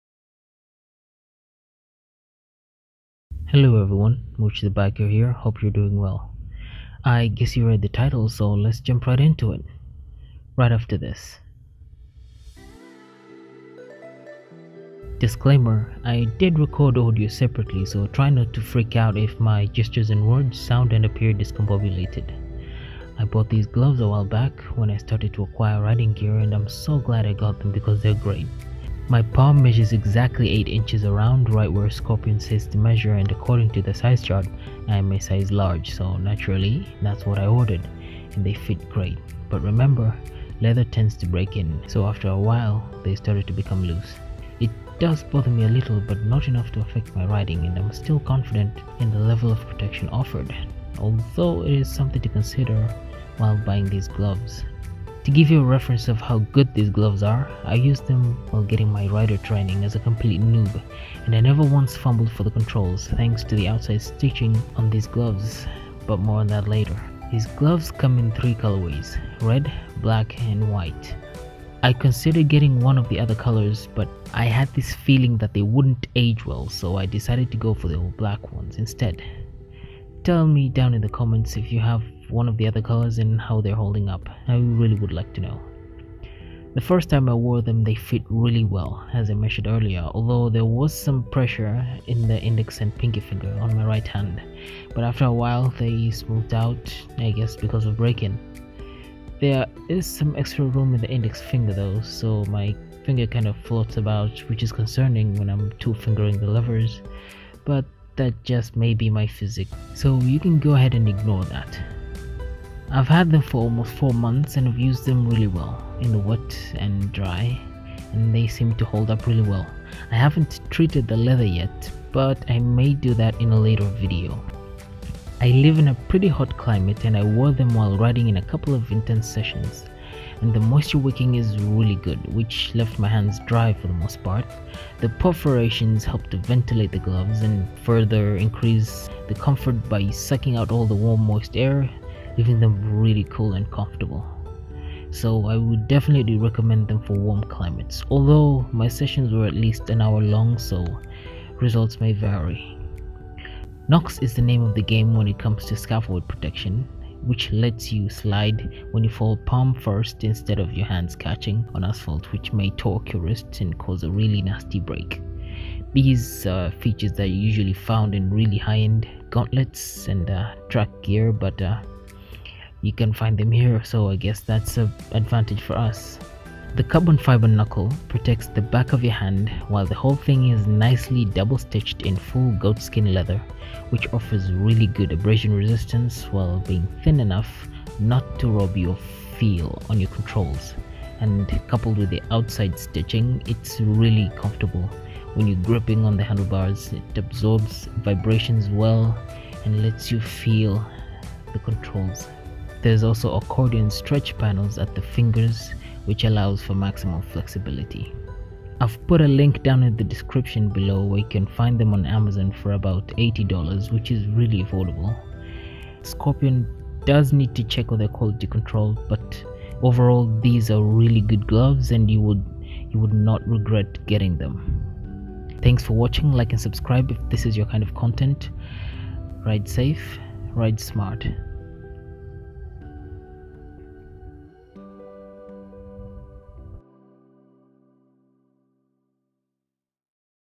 Best Motorcycle Gloves? Scorpion SGS MK II Short Cuff Gloves
This is part of a new series of videos on affordable motorcycle gear. being a new biker I elected to share my experiences to help others avoid the mistakes I made.

The Scorpion SGS MK II Short Cuff Gloves are by far the best affordable gloves I was able to find beating other popular brands at features and price while sacrificing very little.
the features include; carbon fibre split knuckle, Knox palm sliders (improved considerably from the last iteration of the gloves, double stitching for durability and strength, foam reinforced low-side padding for extra abrasion resistance and impact shock protection, foam inserts beneath all hard parts to absorb shock and protect your hands from impacts and vibrations, free-floating knuckle guard for added flexibility and also doubles as a low pressure area paired with a series of perforations in the area to promote the removal of warm moist air to keep the gloves cool and dry.
goatskin leather ensures durability, abrasion resistance and thin construction to promote feel for the controls. velcro straps with rubber backing to allow for a comfortable and secure fit.